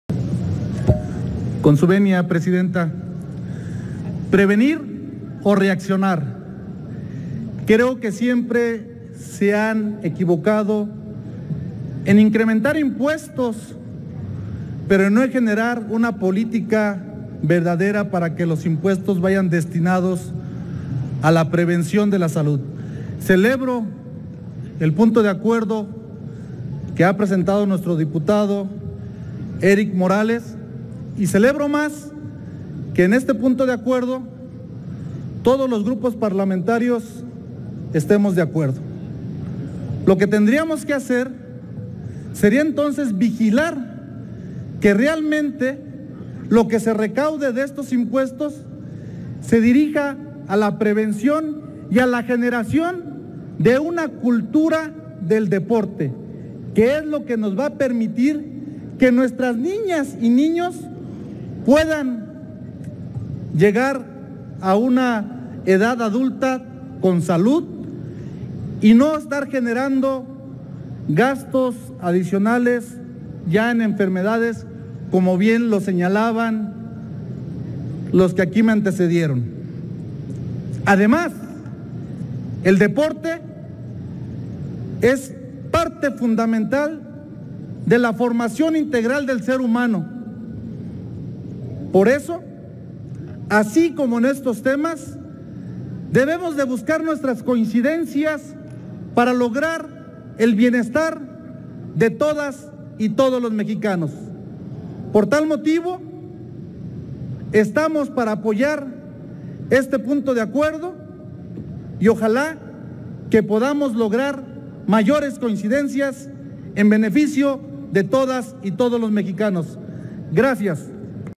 Dip. Ignacio Benjamín Campos Equihua
El Dip. Ignacio Benjamín Campos señala que se han generado los impuestos para diversas causas pero no se han desarrollado las políticas públicas que permitan hacer frente a los problemas de diversa índole. Apunta que el deporte es parte fundamental del crecimiento del ser humano, por lo que es necesario fomentarlo para obtener mejores resultados para la población.